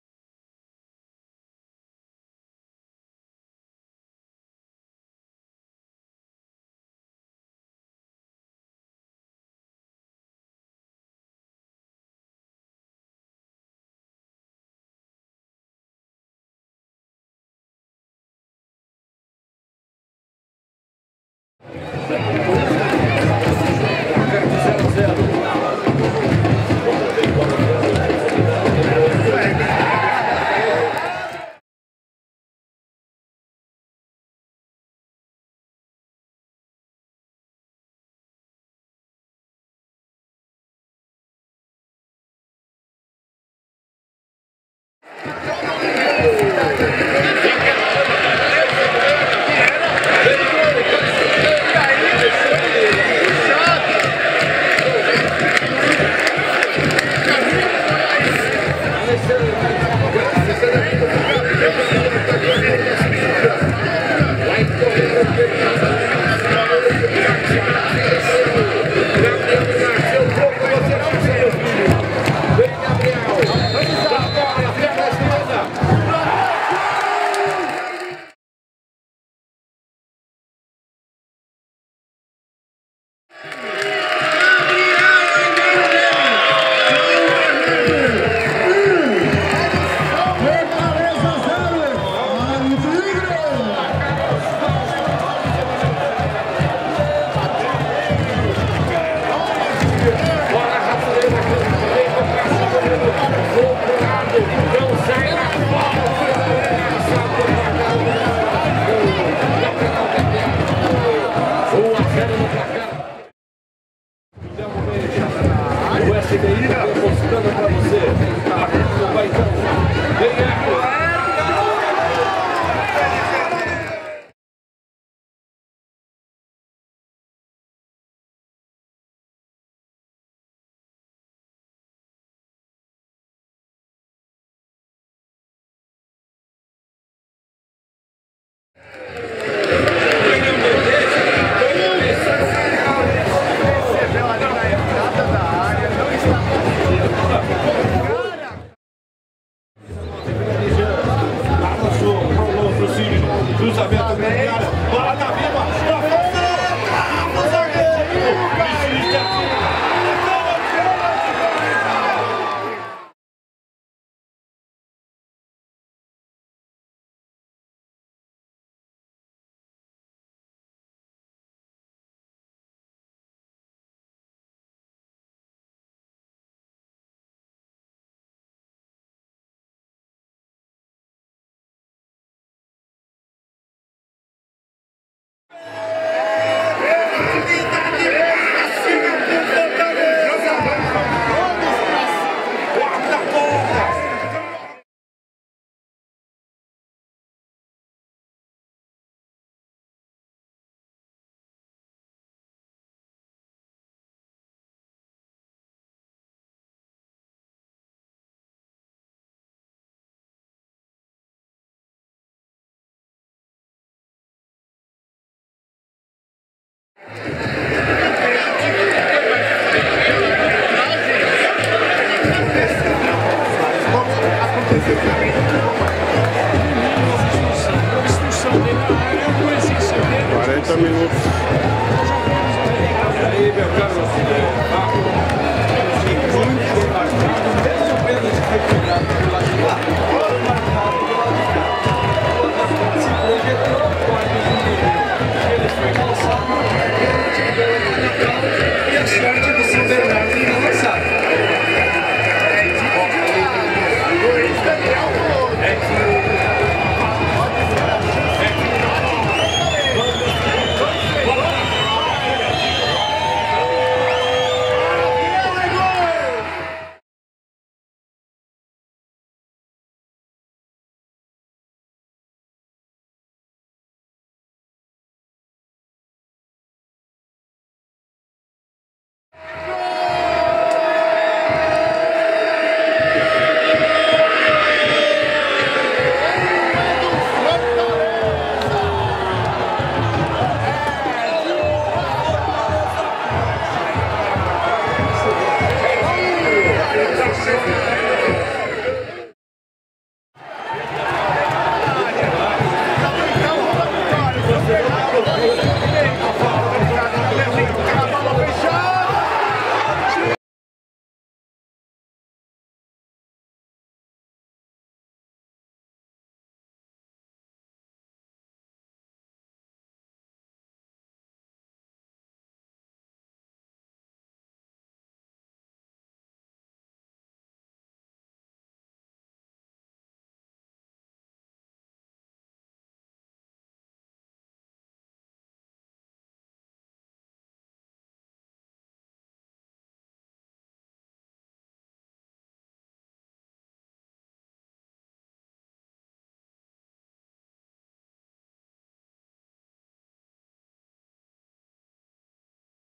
Gols - Copa SP - São Bernardo FC 1 x 1 Fortaleza EC 13 01 13
Opinião: Discutindo a Regra
Após o gol marcado a equipe do Fortaleza foi quase inteira comemorar junto a torcida ficando em campo o goleiro e um jogador próximo a lateral, enquanto o time do São Bernardo dava a "nova saída", sendo a mesma anulada pelo juiz que resolveu esperar a equipe do Fortaleza se "posicionar" para então reiniciar o jogo.
Se, e somente se, qualquer jogador não pode estar fora das quatros linhas do campo por qualquer motivo sem autorização do "arbitro que segue a regra", mas que usa de condescendência quando permitir a comemoração de um gol fora do campo, não deveria o mesmo impedir o reinicio do lance caracterizado como nova saída, portanto nada contra o Fortaleza que em minha opinião foi imprudente em ter nove jogadores fora do campo de jogo, mas o juiz prejudicou o time que estava dentro de campo e como a regra diz que a partir da bola parada o jogo deve ser reiniciado, não é obrigação de se esperar o adversário se arrumar. Como também nem há impedimento, e neste caso não havia por ter um jogador de "linha" dentro do campo a frente da linha da bola, portanto o tigre poderia ter feito o seu gol de empate, ou estou errado? Em todo caso foi o que vi, certo ou errado.